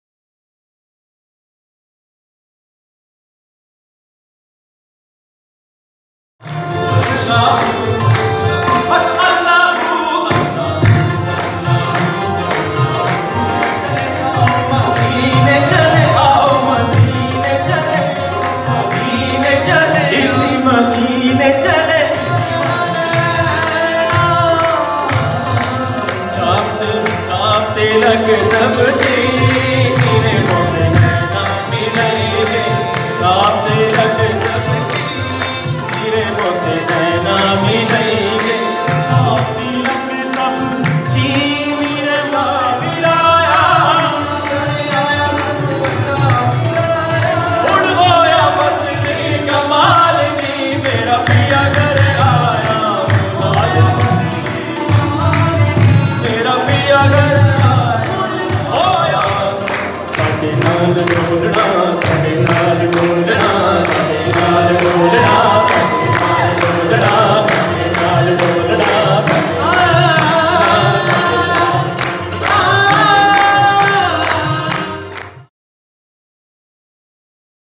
Fahan Shah & Brothers
Fahan Shah & Brothers bring the devotional musical traditions of Quawwali to Adelaide. This mystical form of Sufi music is played from the Middle East through many parts of India and South Asia. Featuring the vocals of Fahan Shah (Karachi) with his group of musicians and singers including Chetan Ramlu (Wellington), Ravi Madhawan (Melbourne), Kuldeep Bains and Ram Laal vocalists (Sydney), and Keith Preston (Adelaide) on the Kashmir Santoor. The group carries on the ancient wisdom of Sufi Qawwali music, a wisdom that is not only heard but felt. They embody a universal message of tolerance and devotion: